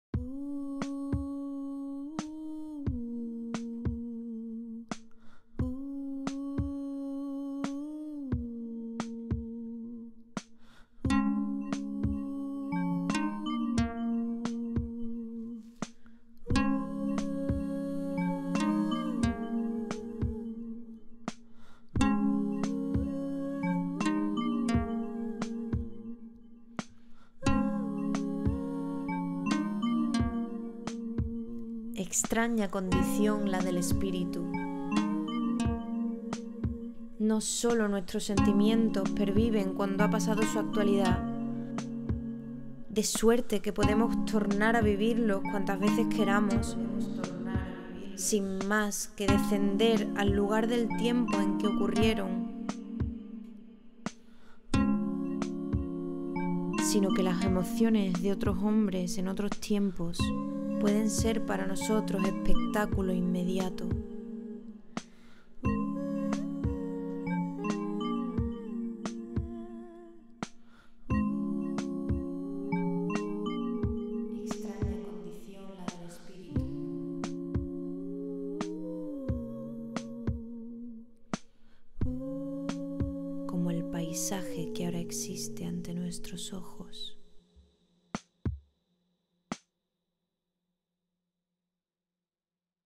El espectador
Se me ha ido la olla. Un poema de Ortega y Gasset (El espectador) y algunas imágenes random de mi galería. Feliz San Valentín, queridas.